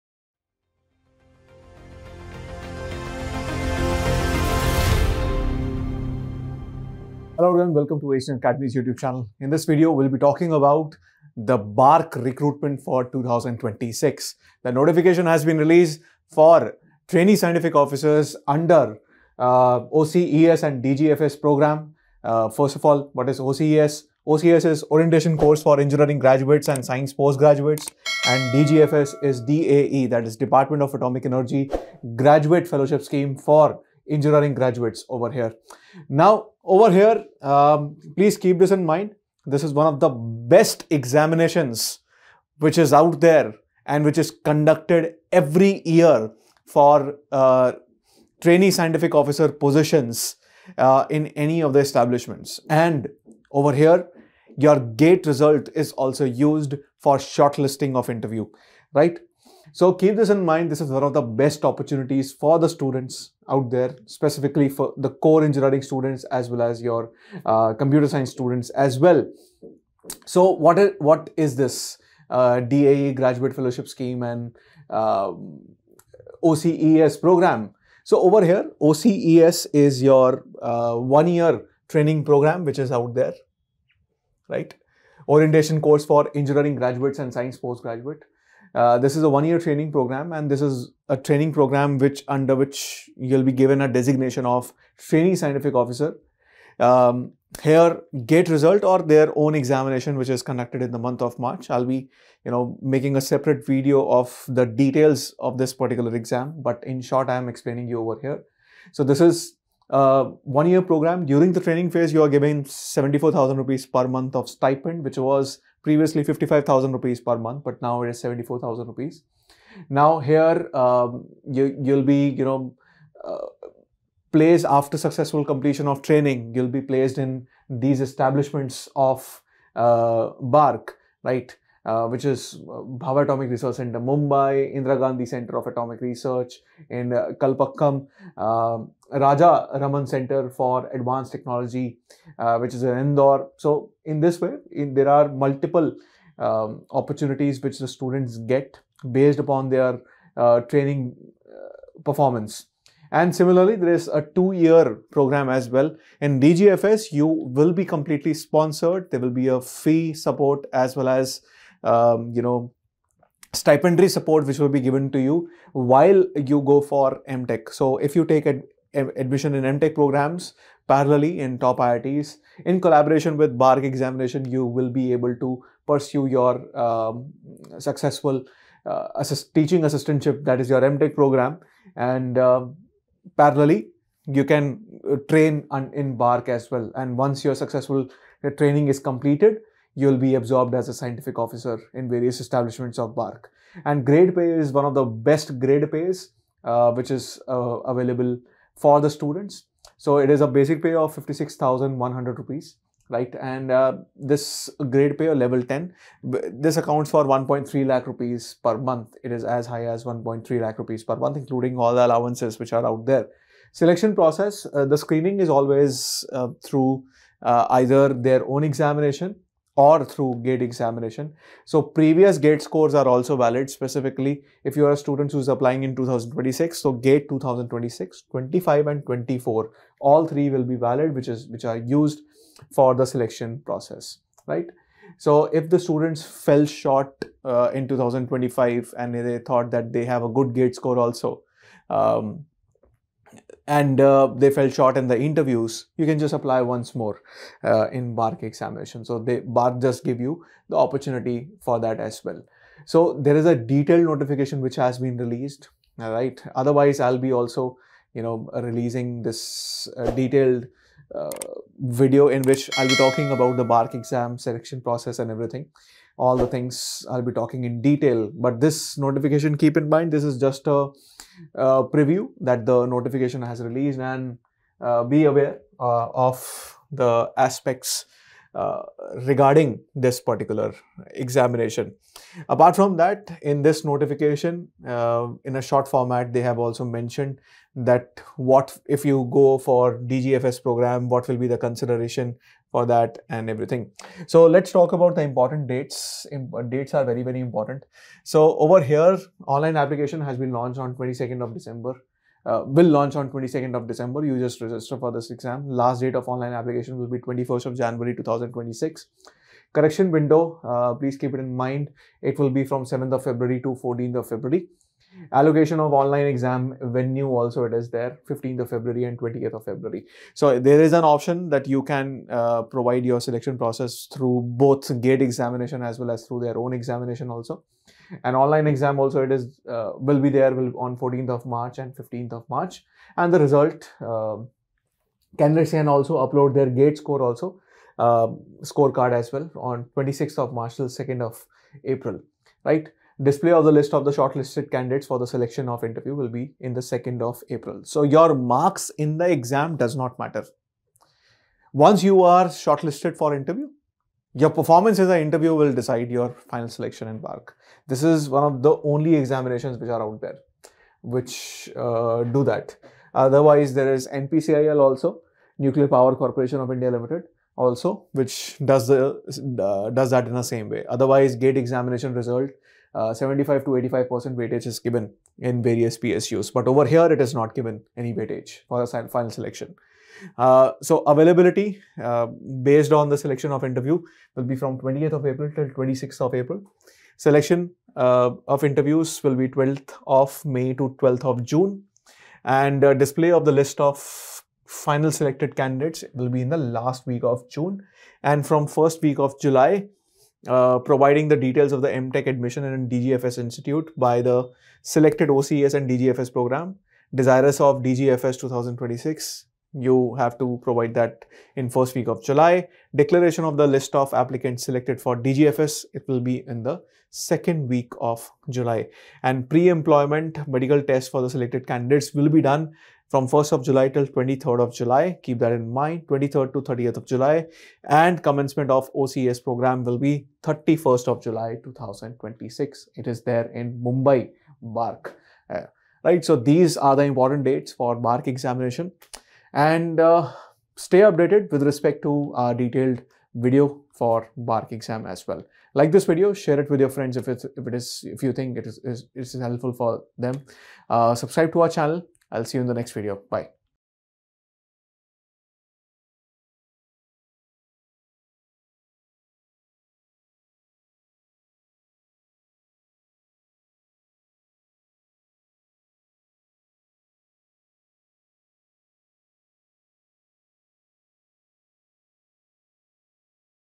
BARC Recruitment 2025 | Scientific Officer | Through GATE 2024/2025/2026 Score | ACE Online
Explore the BARC Recruitment 2025 opportunity for the post of Scientific Officer through GATE 2024, 2025, or 2026 scores. This session by ACE Online guides aspirants on eligibility criteria, selection process, and preparation tips to secure a prestigious career at Bhabha Atomic Research Centre (BARC). Learn how to maximize your GATE score, understand the recruitment process, and take a step closer to your dream government job.